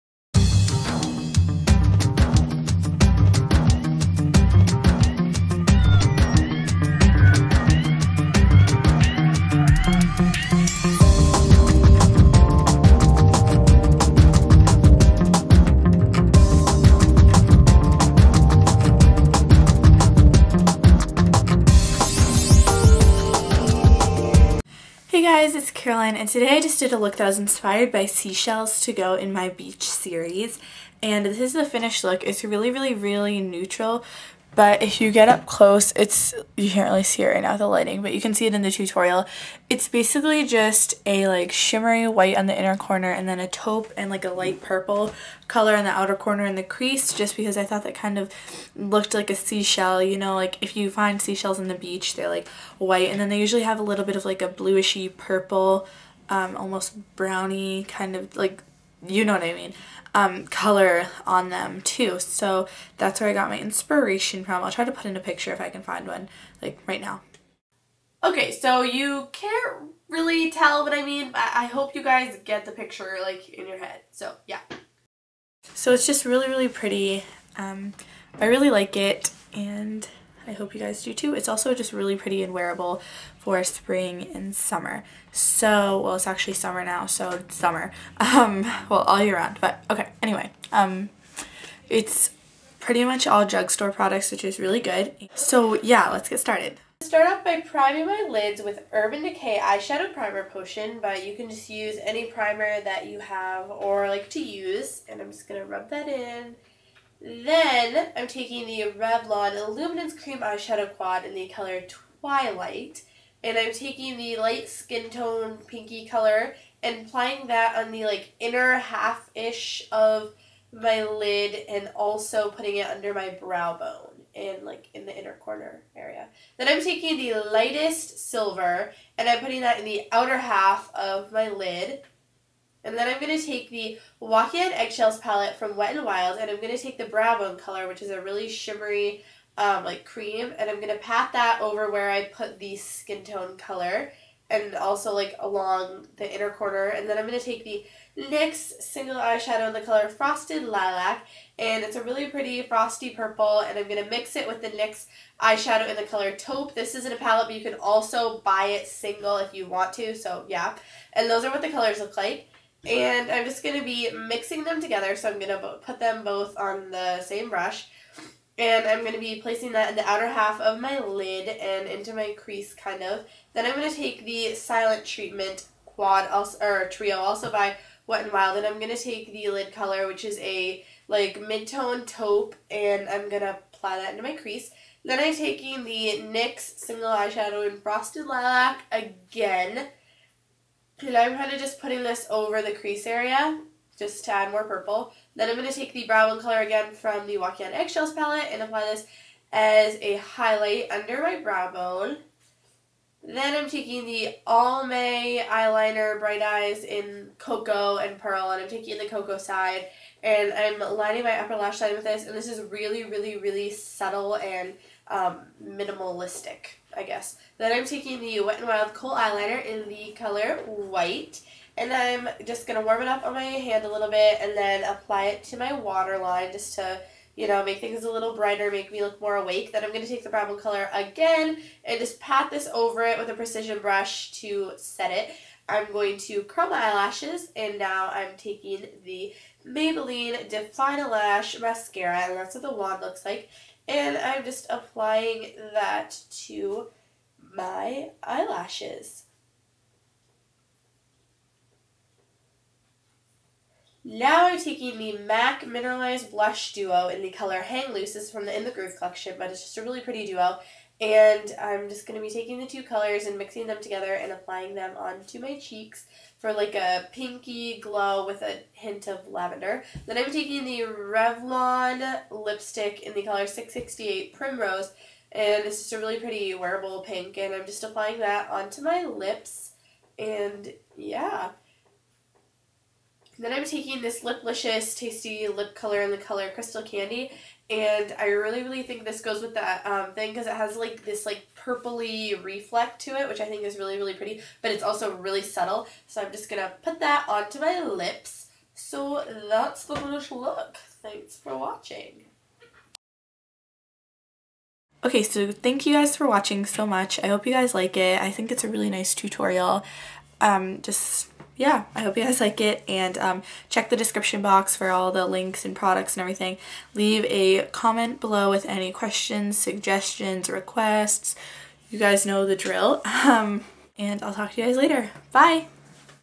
"She Sells Seashells" Part of my Beach Series
PLEASE EXPAND

Products Mentioned:

UDPP
Revlon Illuminance Cream Eyeshadow Quad "Twilight"
Wet and Wild "Walking On Eggshells" palette
NYX e/s "Frosted Lilac"
NYX e/s "Taupe"
Wet and Wild "Silent Treatment" palette
Almay Bright Eyes Eyeliner "Cocoa and Pearl"
Wet and Wild Kohl Eyeliner "White"
Maybelline Define-A-Lash mascara
Mac Mineralize Blush Duo "Hang Loose"
Revlon Super Lustrous Lipstick "Primrose"
Liplicious Tasty Lipcolor "Crystal Candy"

Nail Polish: OPI Koala Bear-y and OPI Pinking of You

Shirt: American Eagle

I'm an NW15 from Mac.

I hope you guys enjoyed this video! Let me know any requests in the comments below.

-Caroline :)

FTC: I paid for everything!